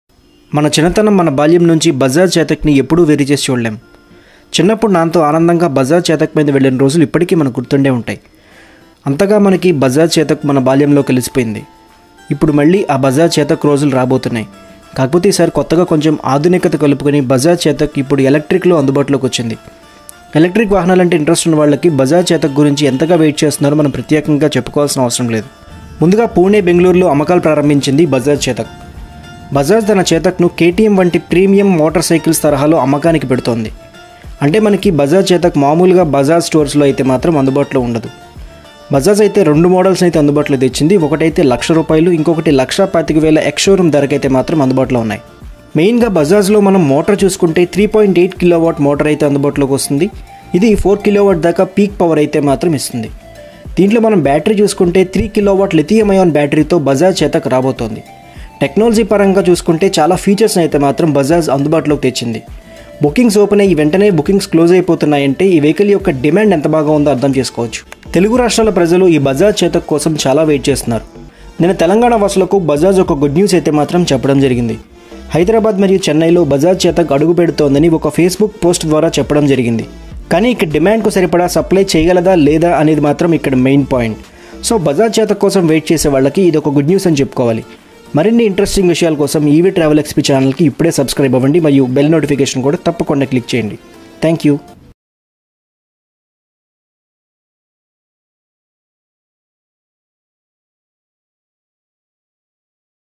Bajaj Chetak In Hyderabad and Chennai soon... | Electric Vehicles | EV TravelXP
Bajaj Chetak to start it's sale in Hyderabad and Chennai very soon..